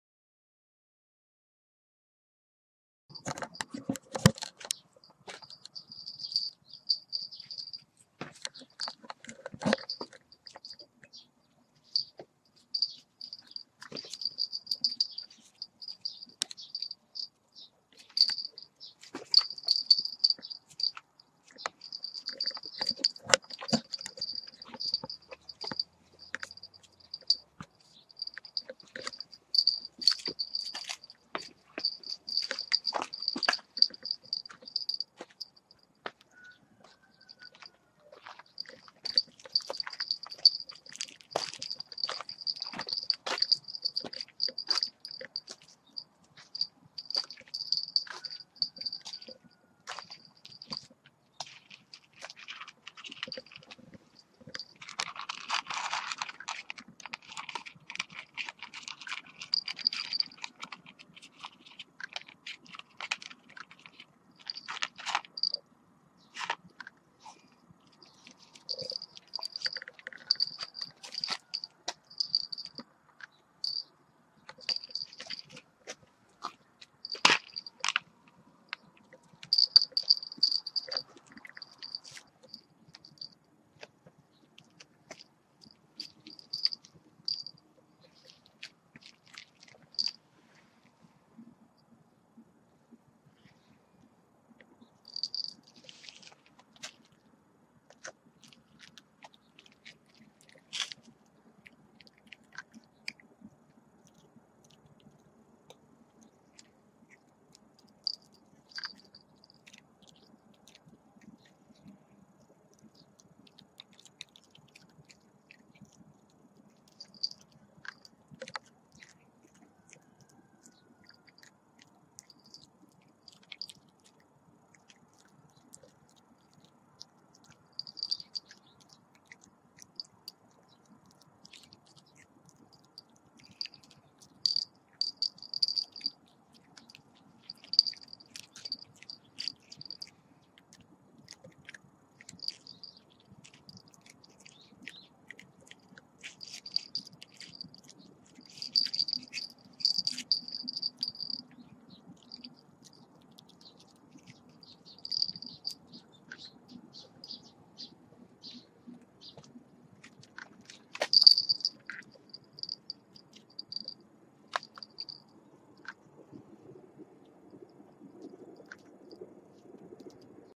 단조로운농장 시즌1] 20화 - 보리가 혼자 노는법. - 꿈꾸는단조 동물농장
누렁이가 떠나서 심심한 강아지 영상입니다.